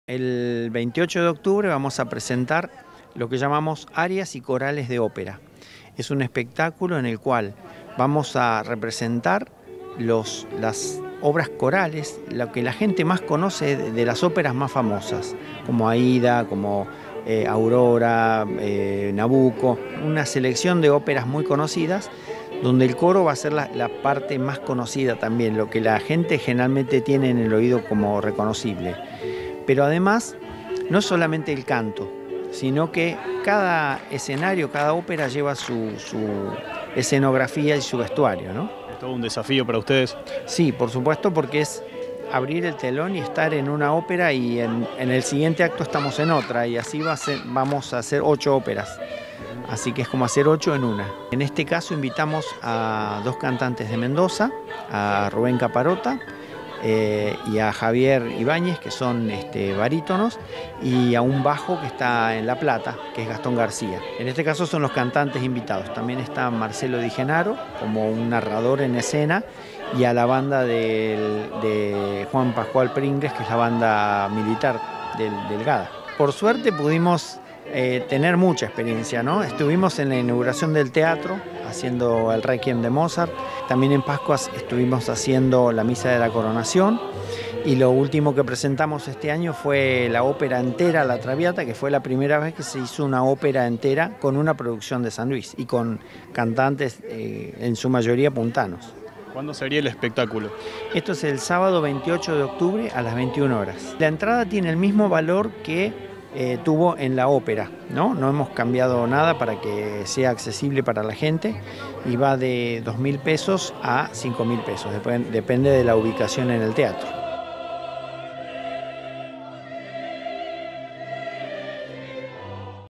Preparan Un Espectáculo De Ocho Operas.
🔴 San Luis Lírica ensaya y prepara un espectáculo de Ocho Operas, con puestas en escena con artistas líricos invitados de otras provincias y la participación especial de la banda de la policía.